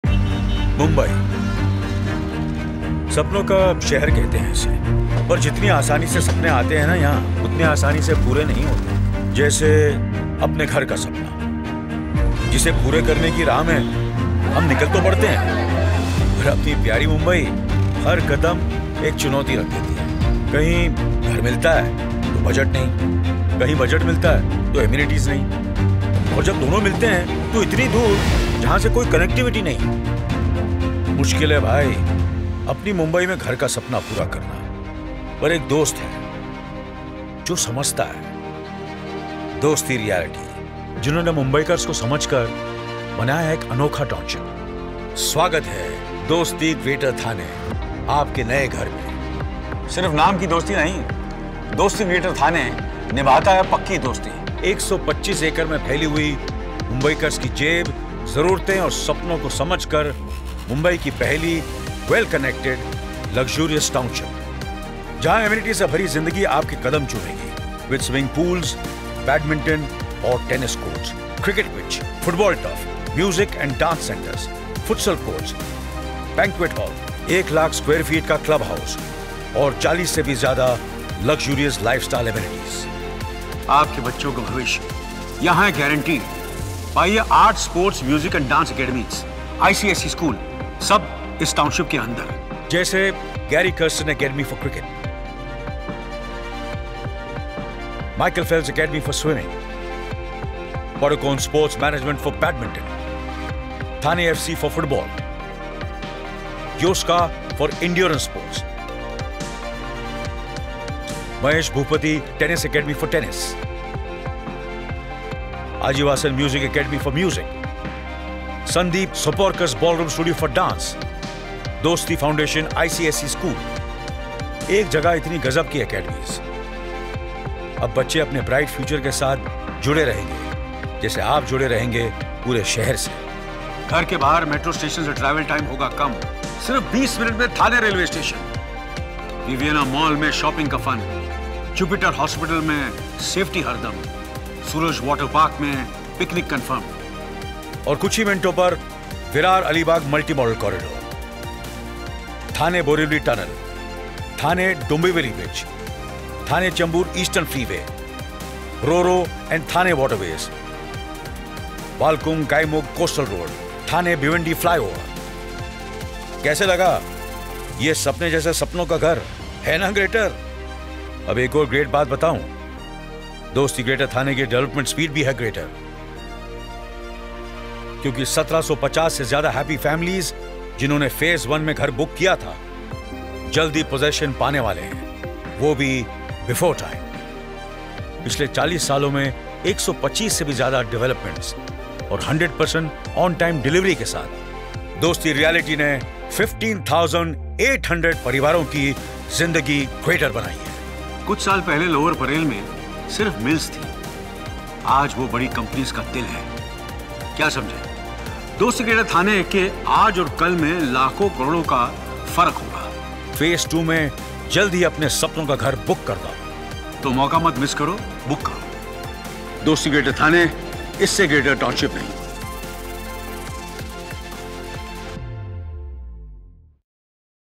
Dosti Greater Thane | IsseGreaterTownshipNahi
Anil Kapoor, the epitome of Bollywood charm, unveils the marvels of  Join the journey where connectivity meets comfort, and amenities redefine luxury.